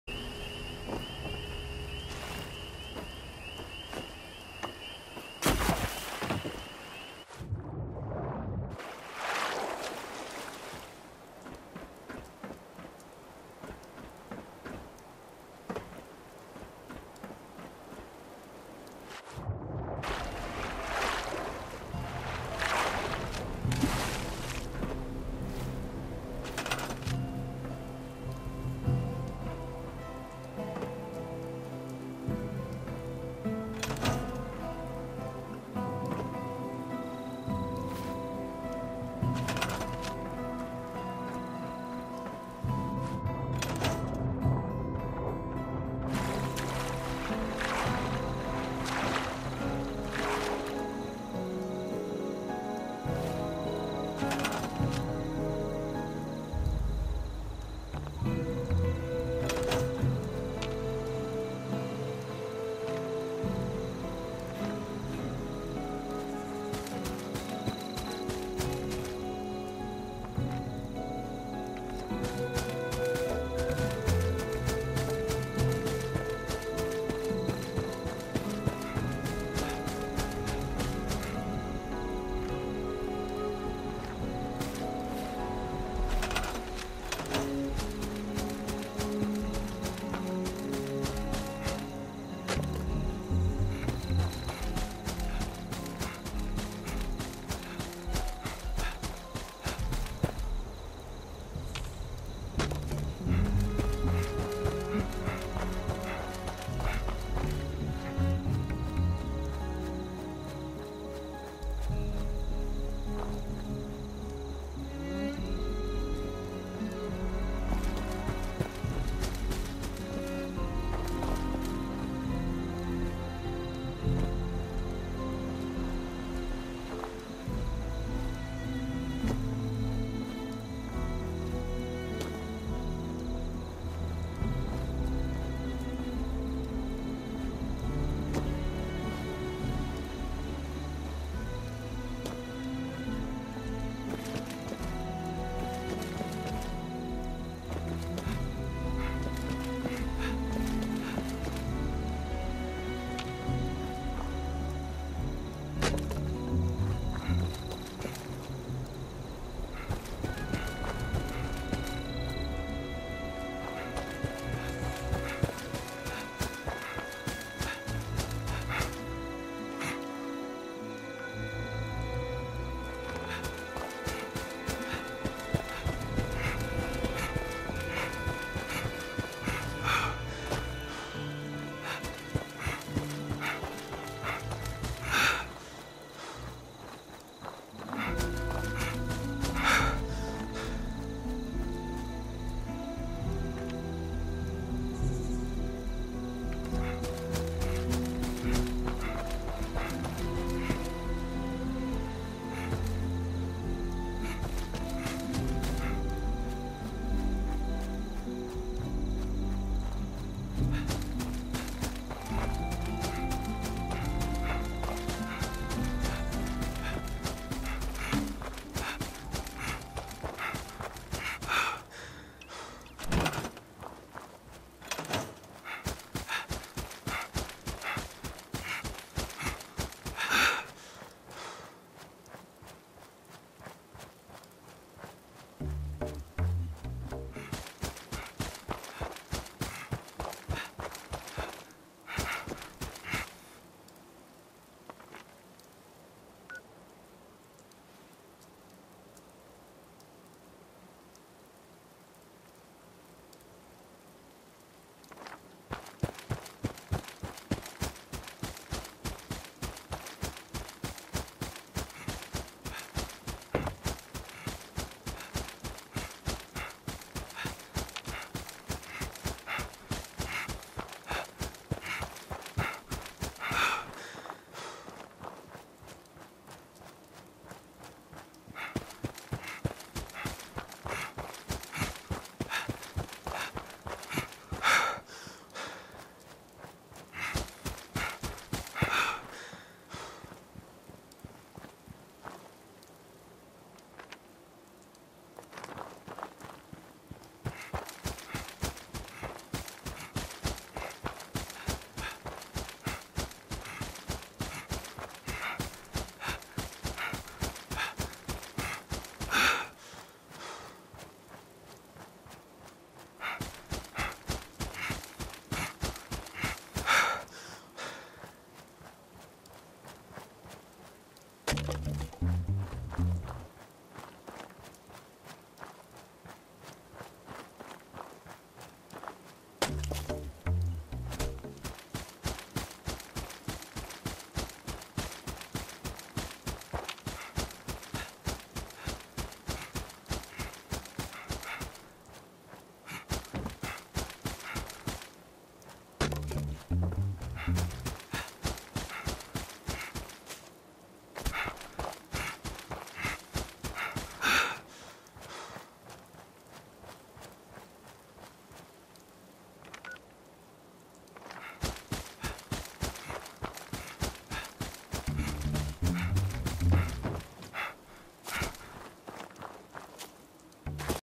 Out Of Bounds
Accidentally broke out of the map while looking for a new CAMP location.  Not much to do there